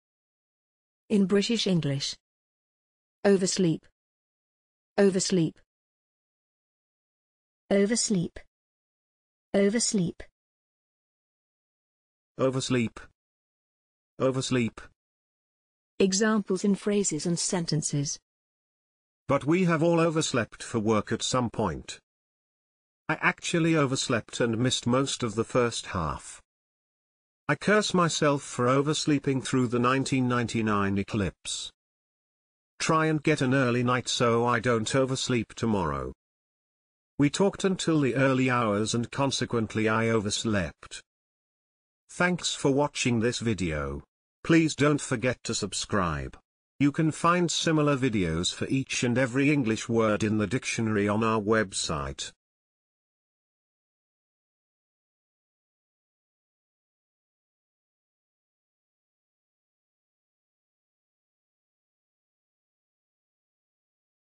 oversleep - pronunciation + Examples in sentences and phrases
This is the pronunciation of oversleep in British English + a few examples of its pronunciation in phrases and sentences.